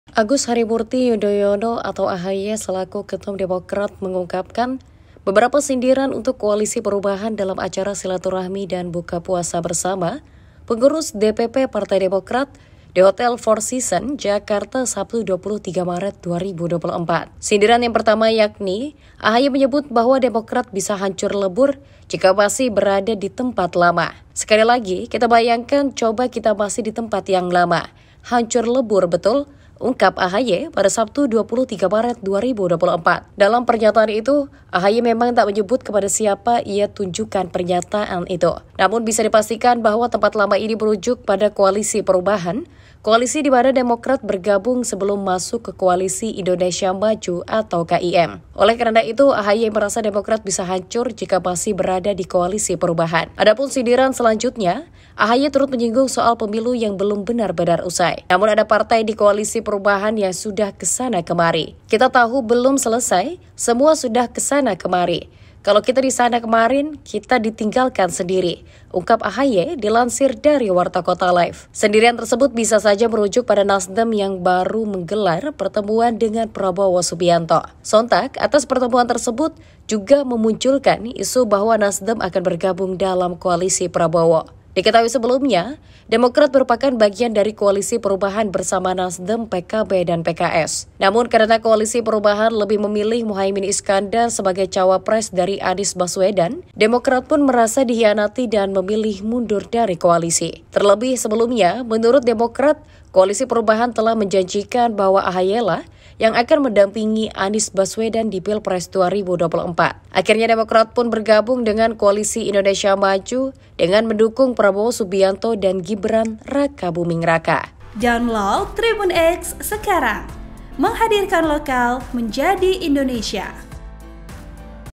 Sindiran Menohok AHY ke Koalisi Perubahan: Singgung Soal Demokrat yang Hanpir Hancur
BANJARMASINPOST.CO.ID - Agus Harimurti Yudhoyono (AHY) selaku Ketum Demokrat mengungkapkan beberapa sindiran untuk Koalisi Perubahan dalam acara silaturahmi dan buka puasa bersama Pengurus DPP Partai Demokrat di Hotel Four Season, Jakarta, Sabtu (23/3/2024).

Sindiran yang pertama yakni, AHY menyebut bahwa Demokrat bisa hancur lebur jika masih berada di tempat lama (Koalisi Perubahan).

"Sekali lagi kita bayangkan, coba kita masih di tempat yang lama. Hancur lebur, betul?" ungkap AHY, Sabtu (23/3/2024).

Dalam pernyataan itu, AHY memang tak menyebut kepada siapa ia tujukan pernyataannya itu.

Namun bisa dipastikan bahwa tempat lama ini merujuk pada Koalisi Perubahan, koalisi dimana Demokrat bergabung sebelum masuk ke Koalisi Indonesia Maju (KIM).

Oleh karena itu AHY merasa Demokrat bisa hancur jika masih berada di Koalisi Perubahan. (Tribunnews)